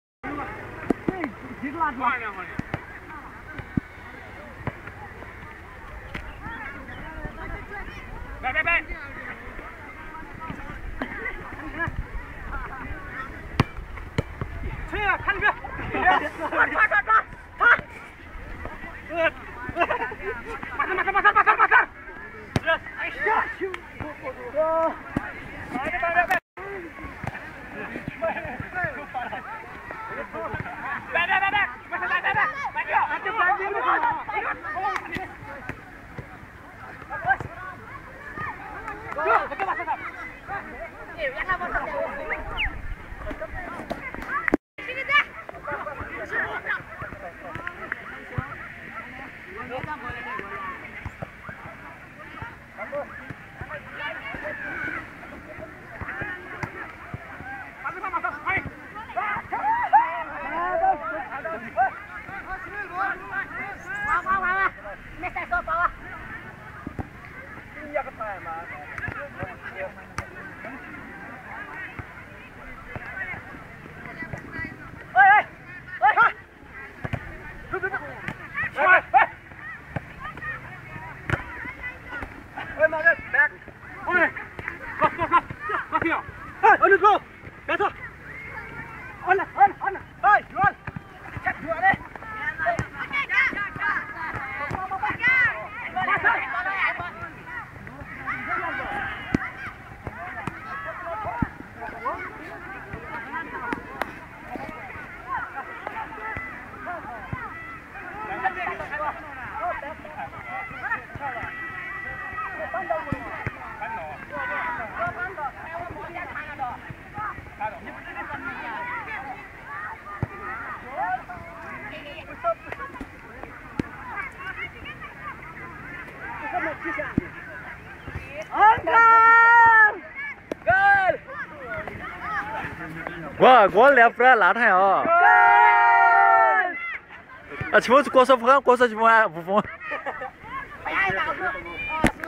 Football Mazic
Football Mazic with Bhulankhel Katos at Madhyapur Thimi, Bhulankhel , Bhaktapur, Nepal.